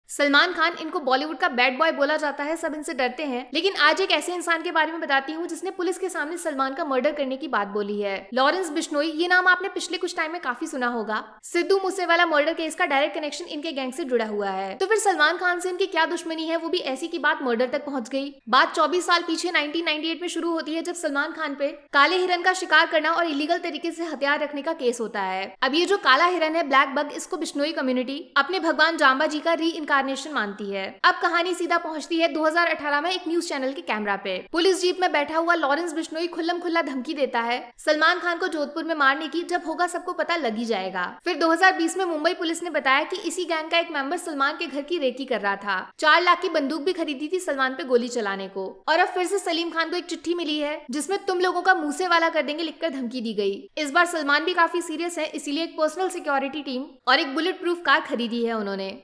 Salman Khan - Lawrence Bishnoi Me Ladai Kyu Ho Gayi?
Salman Khan is one of the Most Popular Bollywood Actor who is known for helping others but has a Grey-Shade personality with him ranging back to 1998 which started the dark chapter. Lekin Asli Panga hai kya aur Salman Sach me Serious hain?